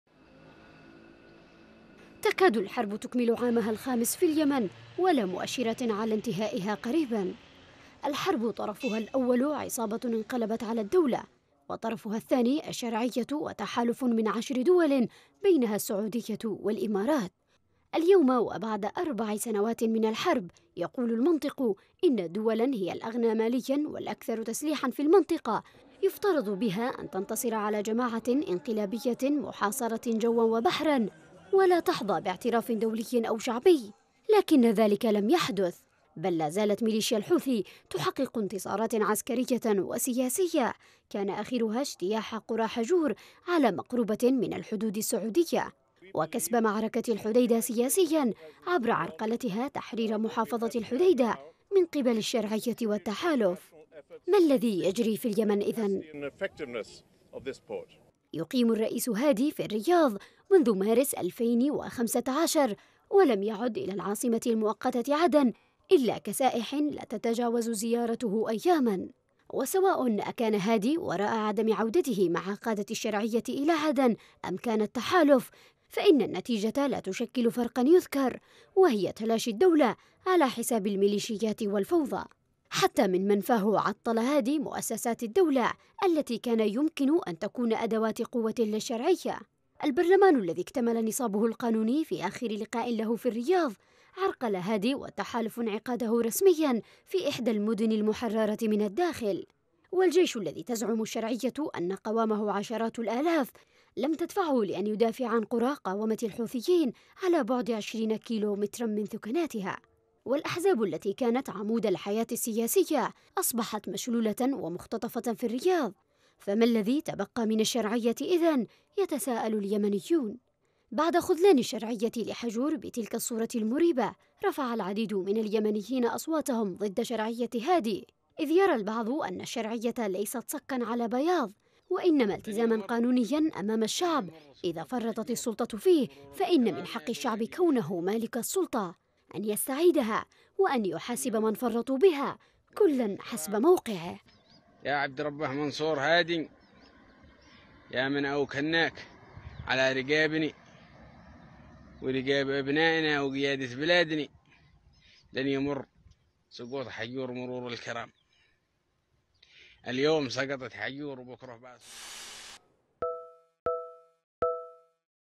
هل يفرط الرئيس هادي بشرعيته؟ | تقرير: أسامة عادل
لم تسقط حجور، بل سقط هادي والاحزاب السياسية والتحالف، هذا ماقاله الكثير من اليمنيين وهم يشاهدون حجور تباد تحت مرأى ومسمع هؤلاء..

في ظلِ هزائمَ عسكريةٍ وسياسيةٍ متوالية.. ماذا تبقى من شرعية هادي وحكومتِه؟
لماذا لا زال هؤلاءِ مقيمين في الرياض حتى الان؟
ولماذا لم يتم تفعيلُ مؤسساتِ البرلمان والجيشِ والأحزابِ السياسية. في المناطق المحررة؟